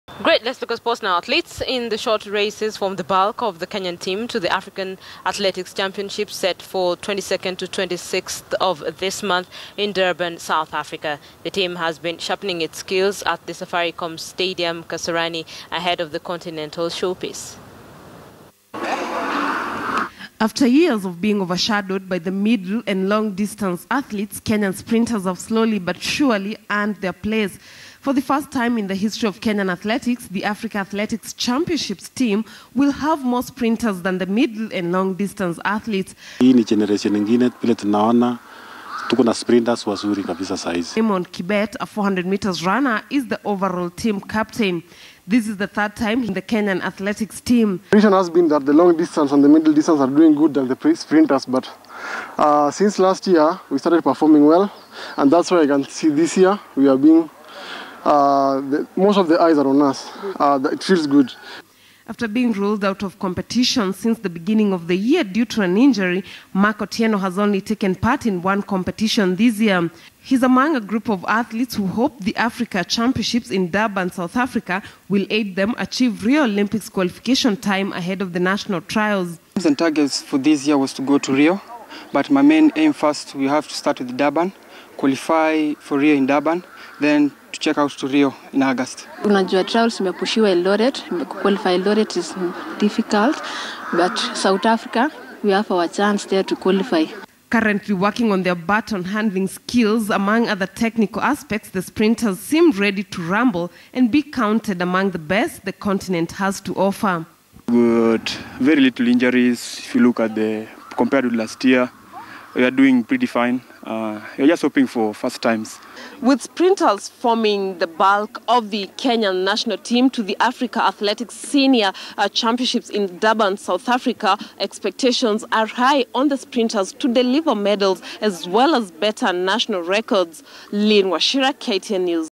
Kenyan team to the Africa athletics championships trains at Kasarani stadium
Athletes in the short races form the bulk of the Kenyan team to the Africa athletics championships set for 22nd to 26th of this month in Durban, South Africa. The team has been sharpening its skills at the Safaricom stadium, Kasarani ahead of the continental showpiece.